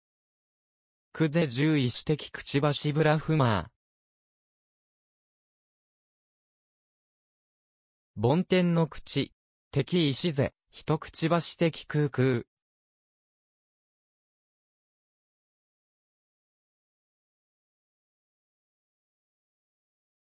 ‘ the mouth of Brahma. ’ meaning
”这句话是什么意思  的嘴Brahma ' ?

 '  梵天之口。 '的意思是：  '人嘴的空腔'